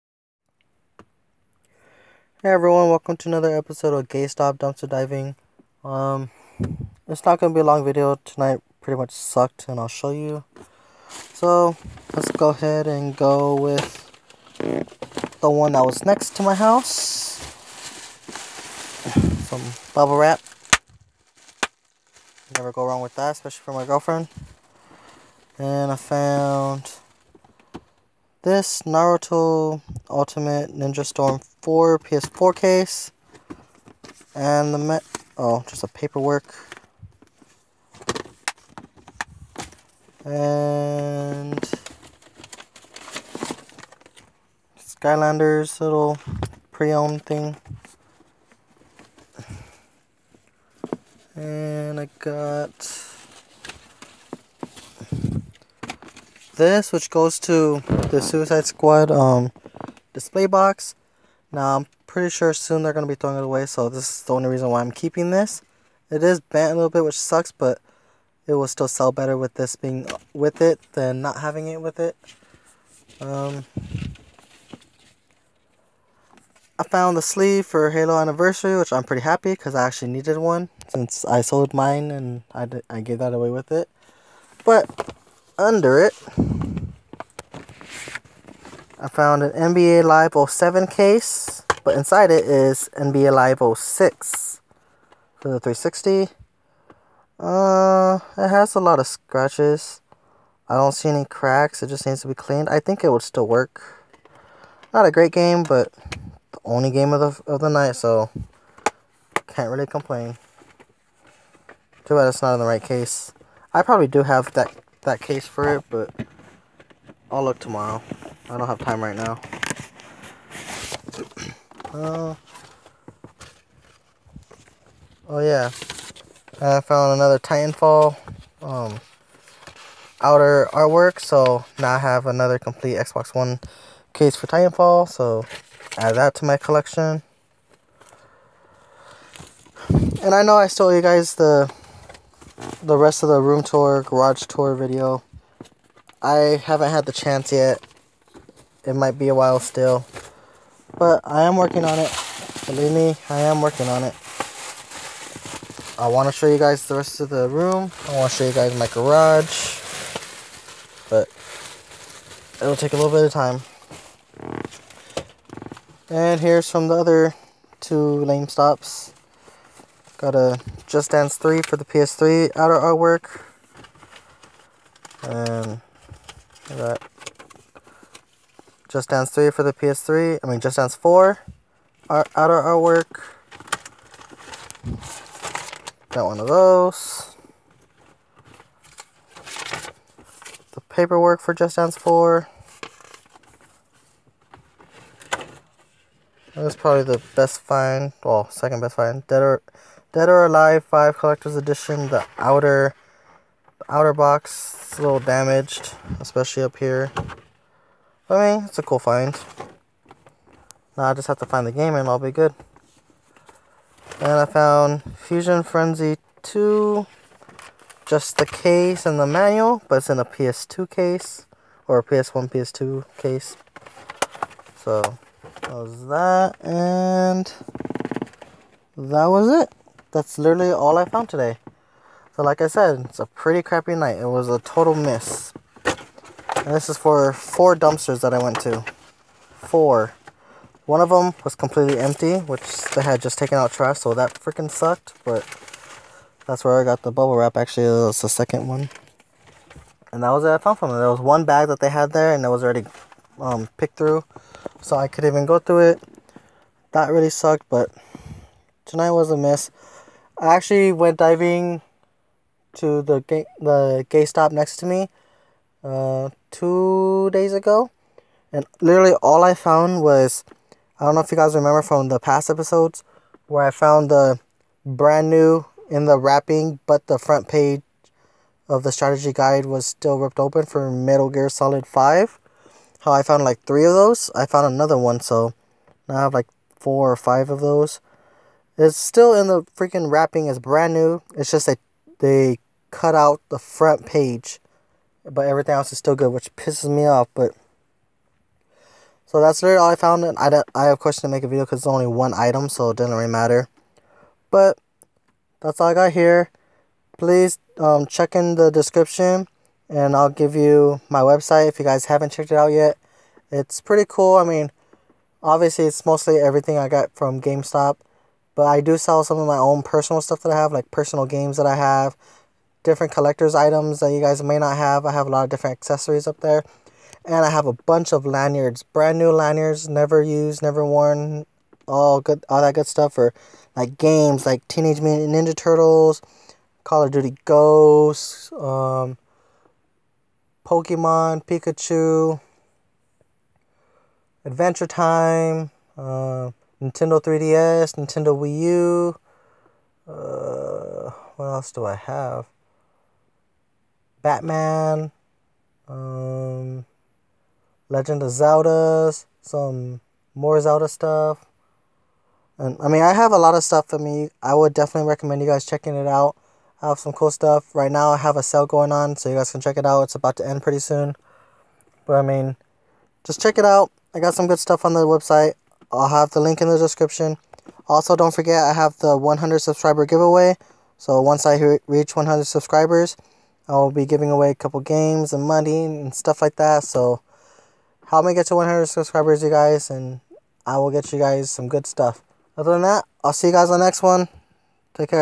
Lamestop dumpster dive ep # 65 not a good start to the week
Not a great start. Barely anything thrown away. 1 decent find though. Hopefully this week picks up.